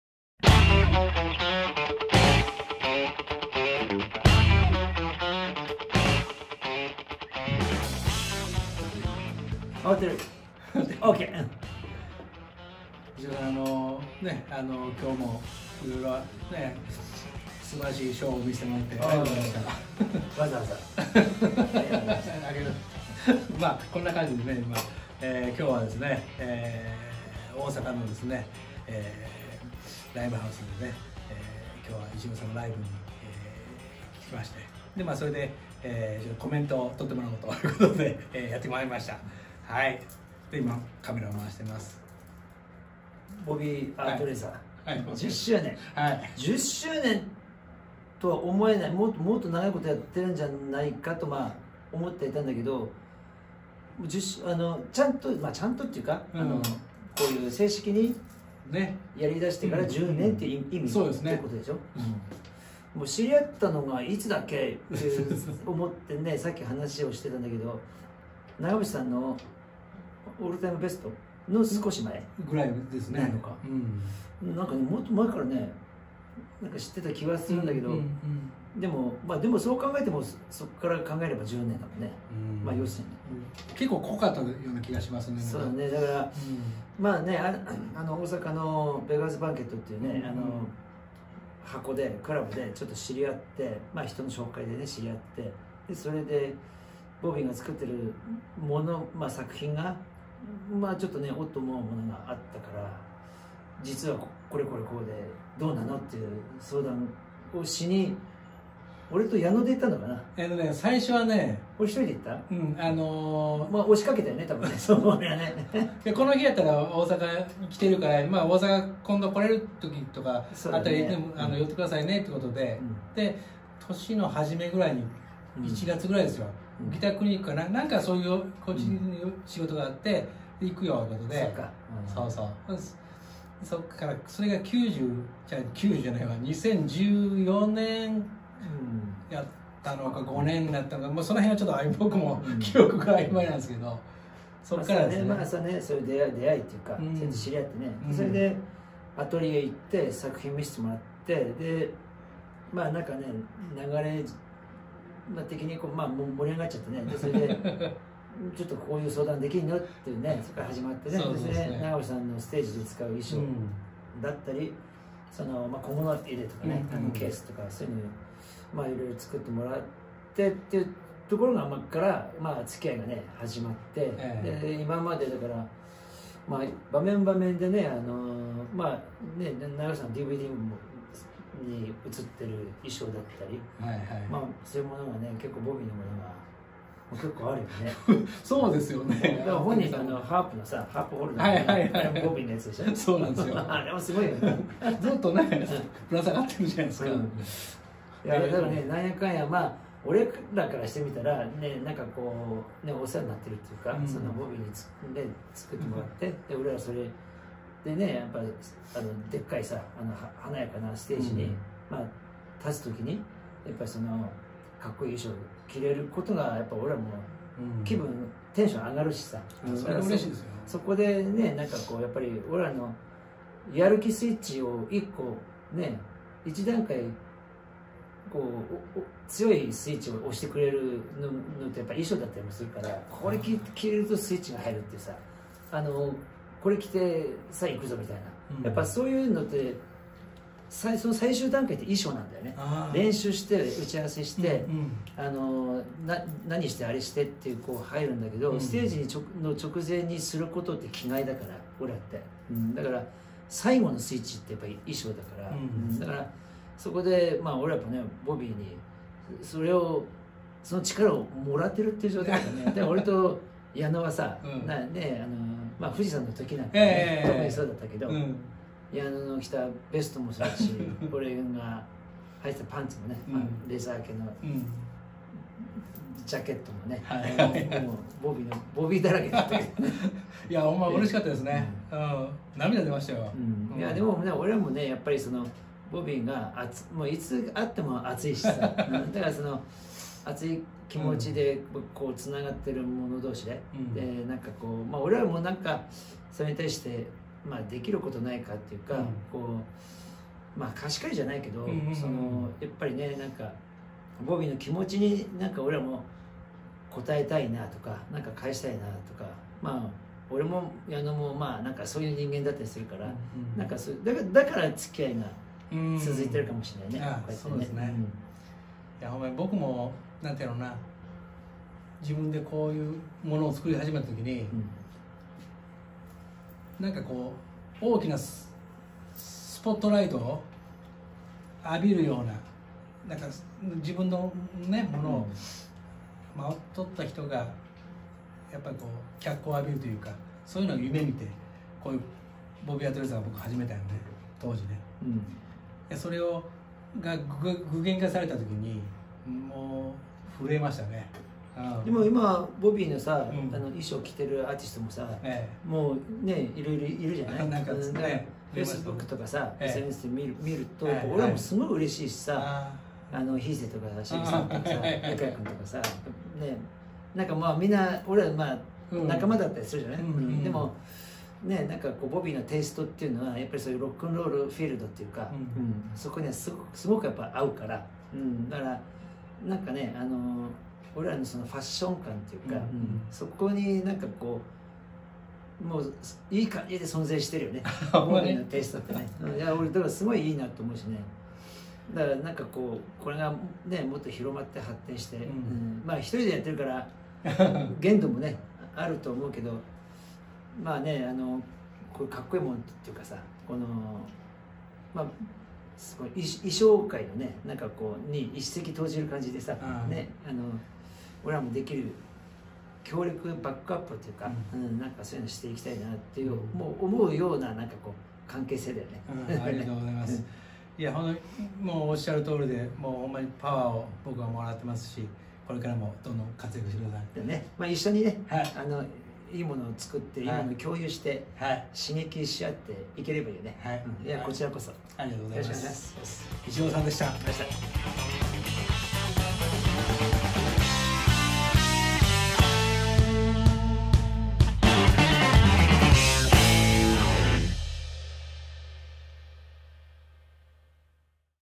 ブルースギタリストichiroさんボビーアートレザー®︎創業10周年スペシャルインタビュー＆対談/Bobby Art Leather ボビーアートレザー
ブルースギタリストichiroさんボビーアートレザー®︎創業10周年スペシャル対談の映像が完成しましたのでシェアさせていただきます。
熱狂のステージを終えたばかりの
ichiroさんとのスペシャル対談をご覧ください♪
詳細はブログにて

収録日：2023.4/16
Mother Popcornにて

ichiroさん、アツいお祝いのメッセージを
コンサートツアーの成功を心から願っています🎸